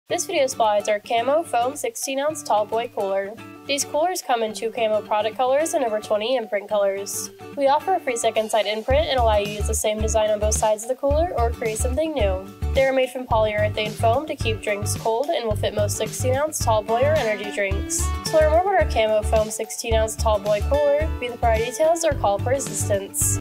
Camo Foam 16 oz. Tall Boy Cooler | TCC-111S-CAMO
These Camo Foam 16 oz. Tall Boy Coolers make the perfect promotional tool or giveaway for any event! Chose from 2 different camo design options and customize with your unique logo or message. These insulators comfortably fit 16 oz. longneck bottles or cans.